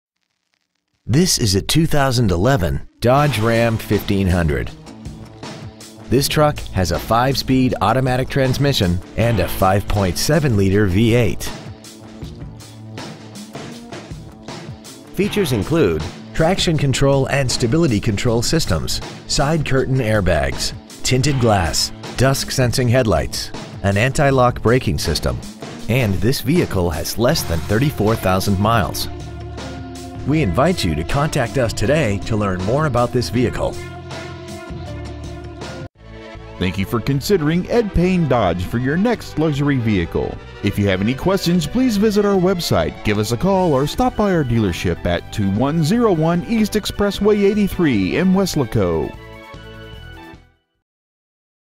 2011 Dodge Ram 1500 Corpus Christi TX
We have been honored to serve the Weslaco TX area , we promise that your experience at our dealership will exceed your expectations ! Year : 2011 Make : Dodge Model : Ram 1500 Engine : 5.7L V8 Trans . : Automatic 5-Speed Exterior : Bright Silver Metallic Clearcoat Miles : 33,580 Interior : Dark Slate/Medium Graystone w/Heavy Duty Vinyl 40/ Stock : T514995 Ed Payne Dodge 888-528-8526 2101 E. Exp 83 Weslaco , TX 78596